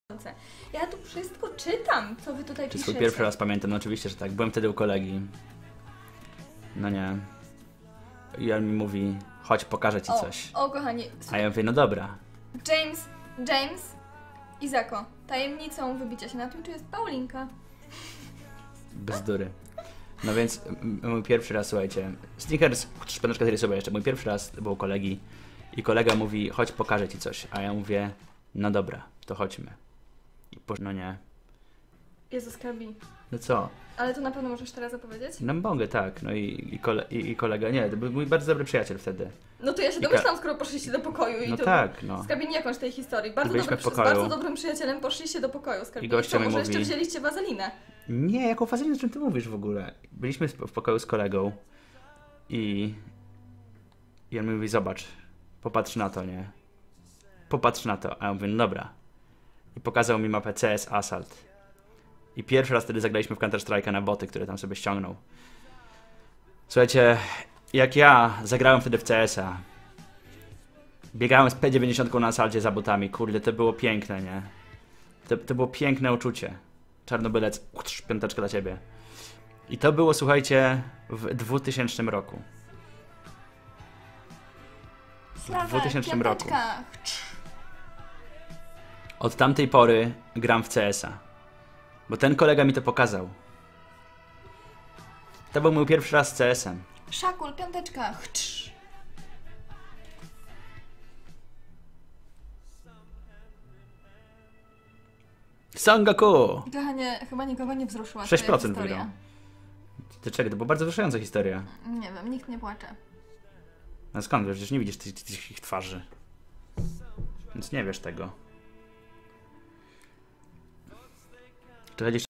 PIERWSZY RAZ IZAKA
IZAK OPOWIADA NA STREAMIE SWÓJ PIERWSZY RAZ , KTÓRY ODBYŁ SIĘ W WIEKU 12LAT